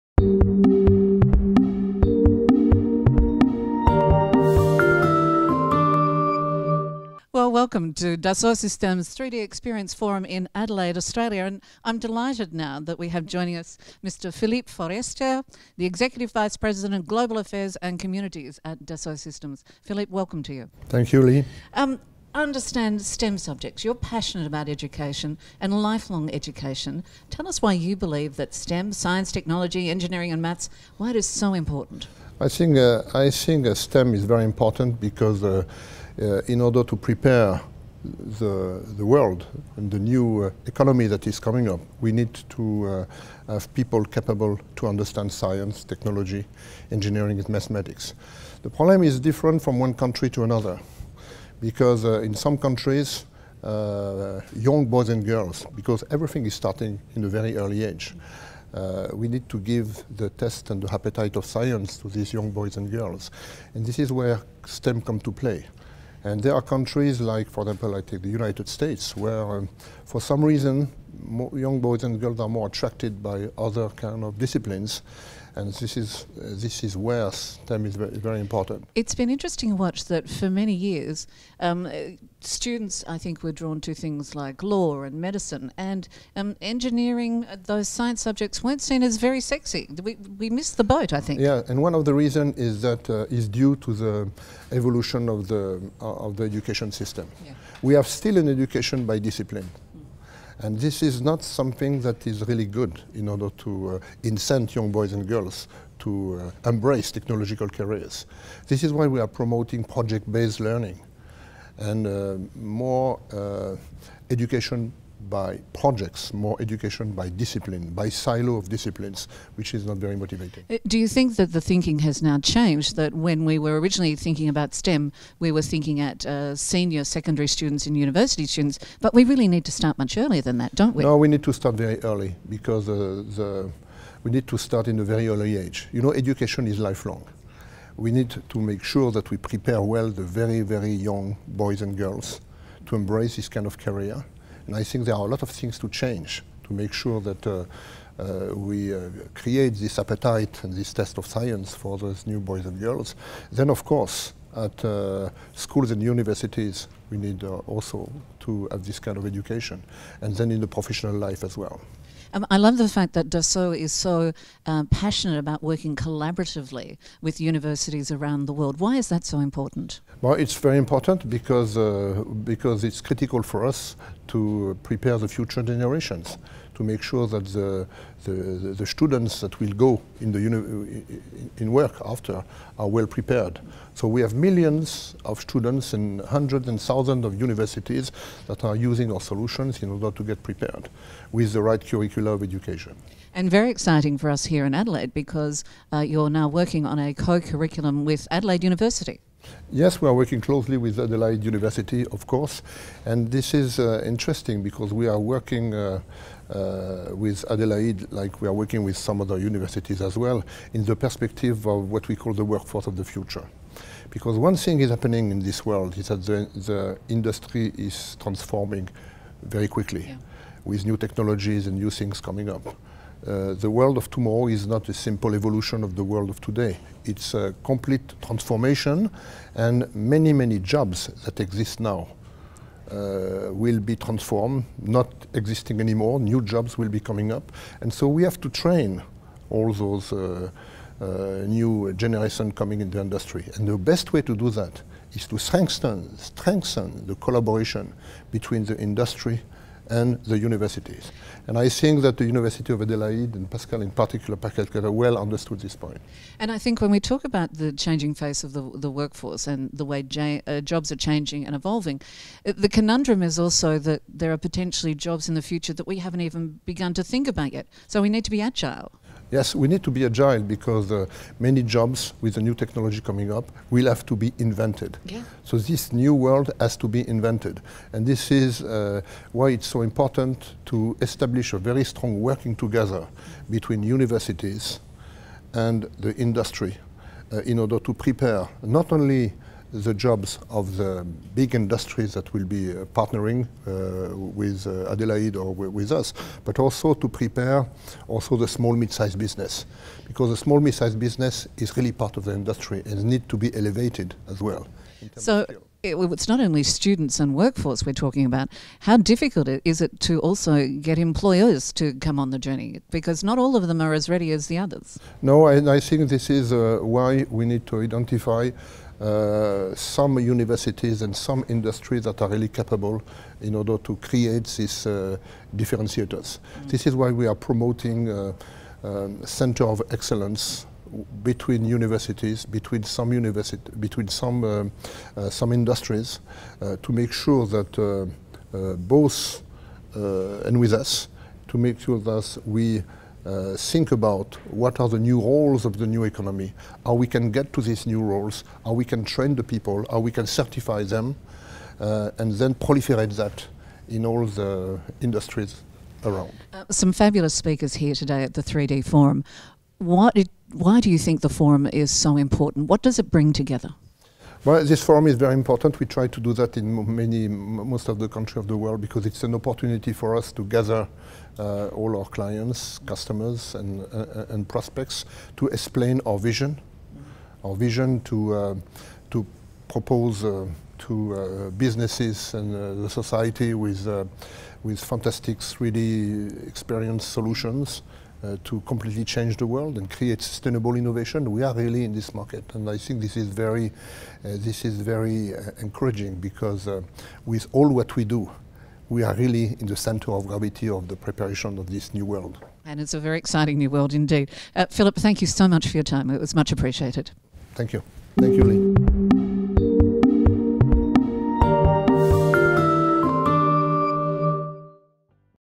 3DEXPERIENCE Forum Asia Pacific South 2018 - Interview with Philippe Forestier - Dassault Systèmes
3DEXPERIENCE Forum Asia Pacific South 2018 - Interview with Philippe Forestier - Dassault Systèmes. 3DEXPERIENCE Forum Asia Pacific South 2018 - Philippe Forestier, Dassault Systèmes' Executive Vice President, Global Affairs & Communities, speaks about the importance of STEM education at an early age, as well as in lifelong education, to ensure a strong pipeline of engineering and technology talent to meet the needs of future industries.